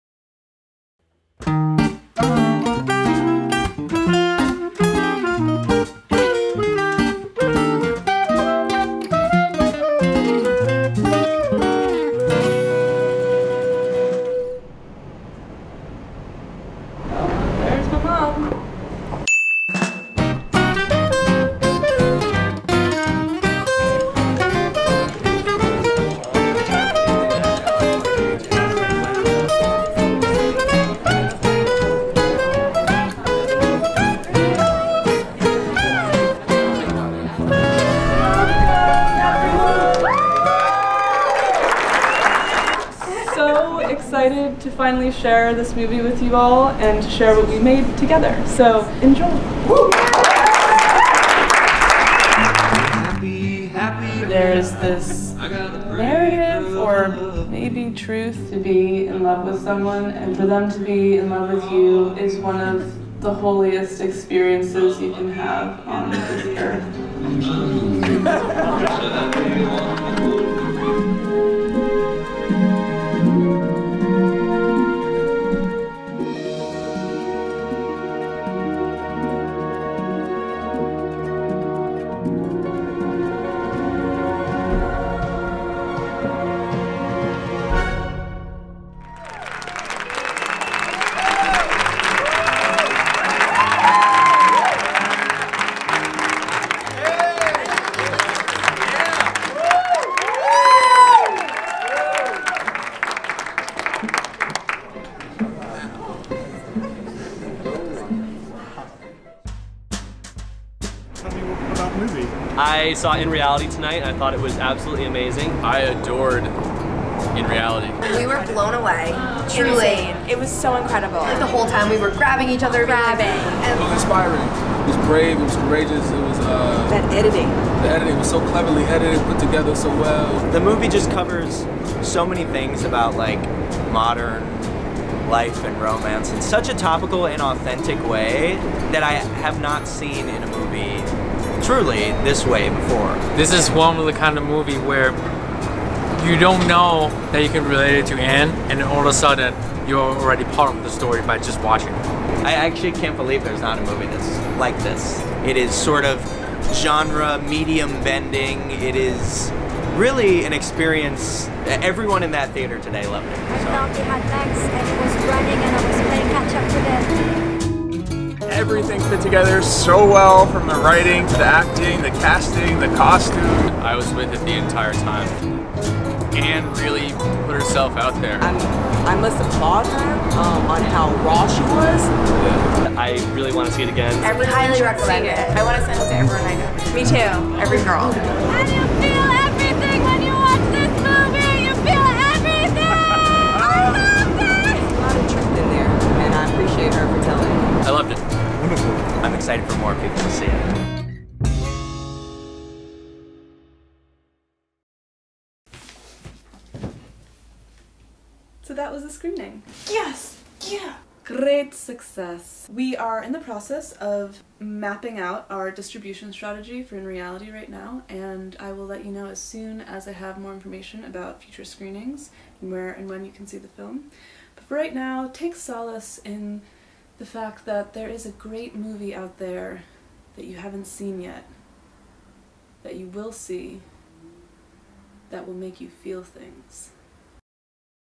AN EVENING AT THE CINEMA
We had our first private screening of my feature film last week and here's how it went..

Music in this video was made entirely by my dear roommate and friend Jon Degen